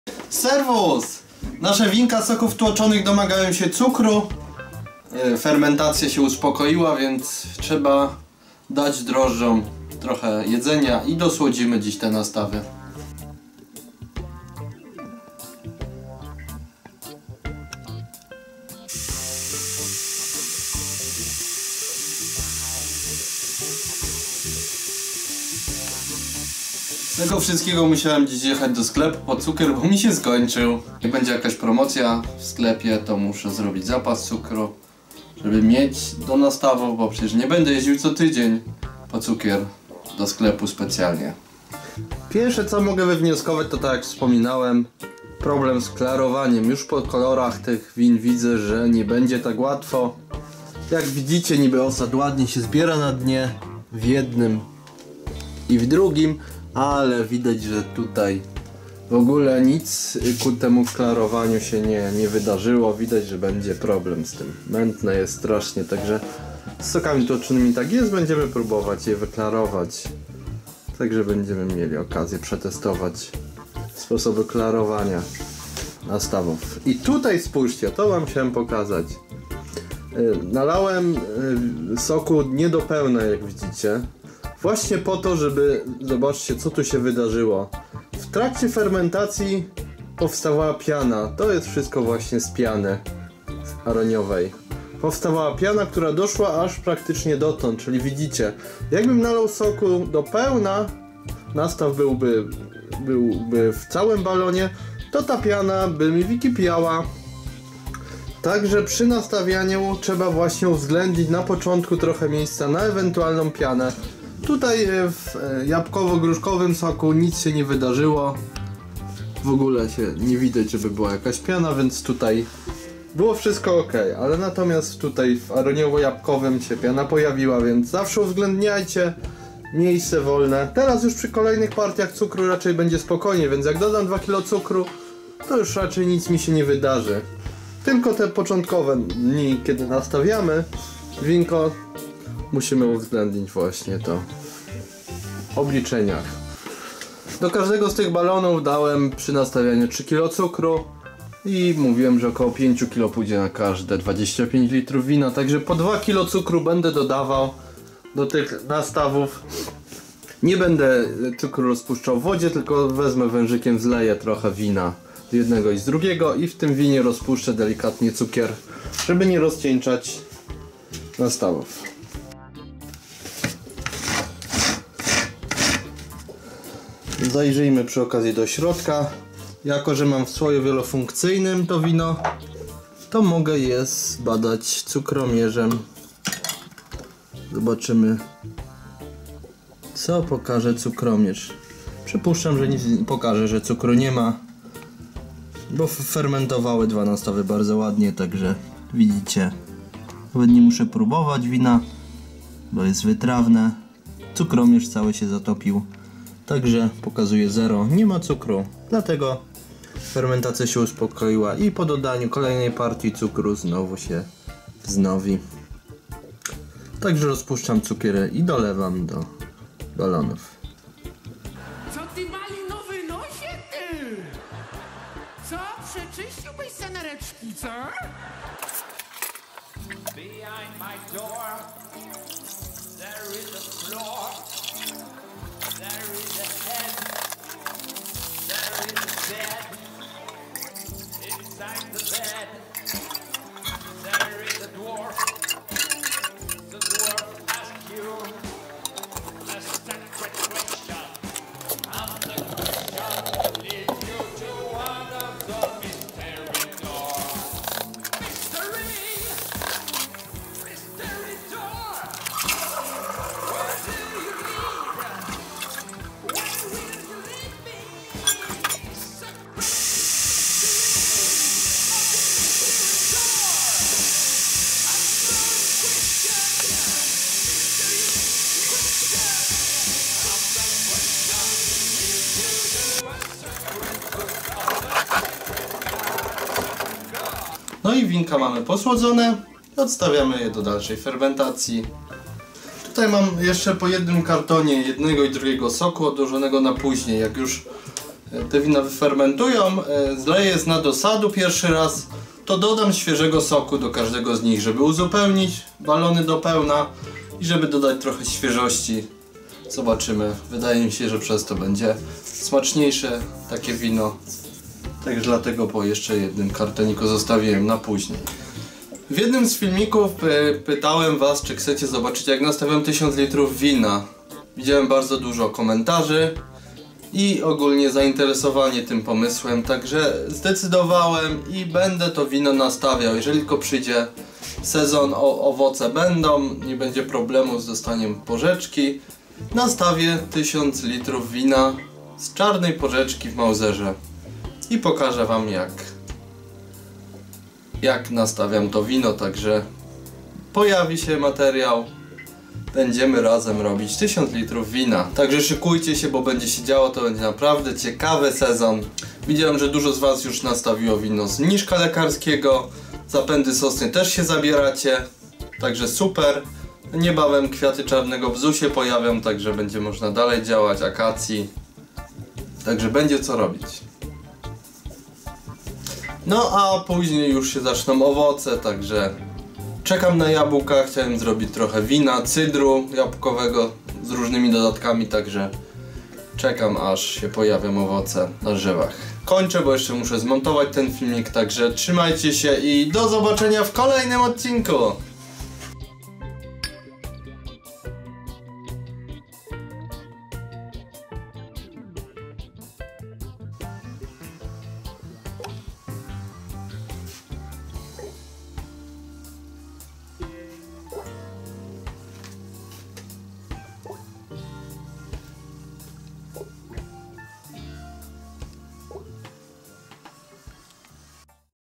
Wino, cydr z Soków Tłoczonych 100% NFC - 1000l wina?!?! DOSŁADZANIE - ekspresowy przepis cz.2
Serwus malinowe nosy ! :) W tym filmiku opowiemy Wam jak wygląda sytuacja z winem z soków tłoczonych!

Zapraszamy na stronę gdzie znajdziesz wszystkie sprzęty i akcesoria produkcji wina, piwa i destylatów.